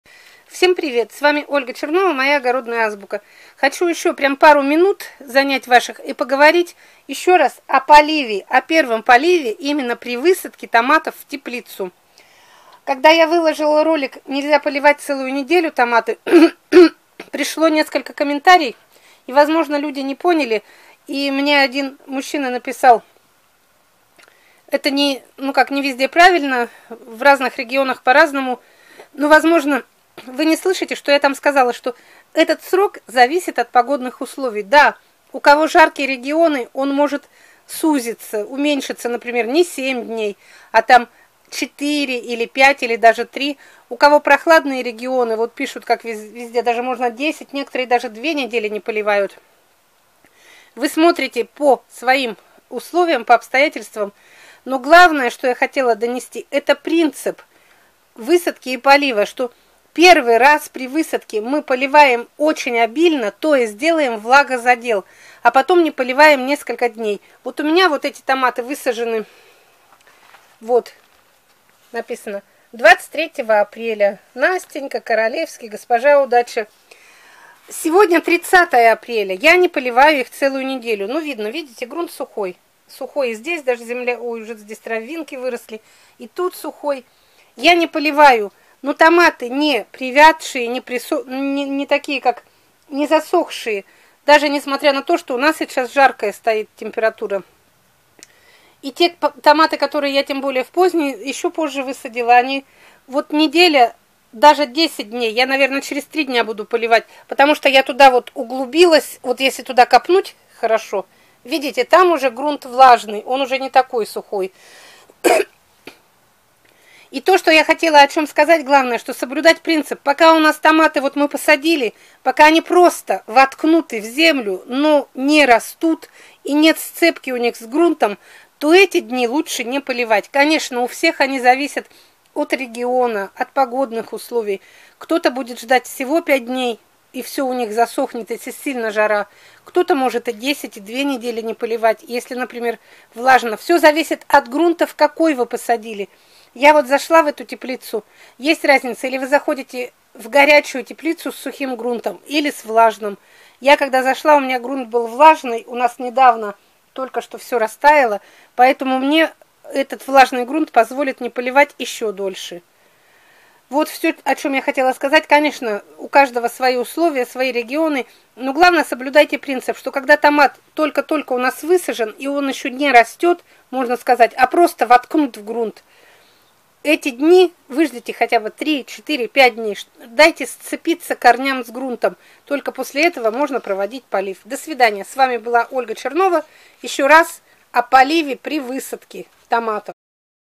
ЕЩЕ РАЗ О ПОЛИВЕ ТОМАТОВ ПОСЛЕ ВЫСАДКИ В ТЕПЛИЦУ. Ольга Чернова.
Видео снято в дополнение к ролику "НЕЛЬЗЯ ПОЛИВАТЬ ТОМАТЫ ЦЕЛУЮ НЕДЕЛЮ!" Многие не услышали, что срок зависит от погодных условий и может меняться в зависимости от региона.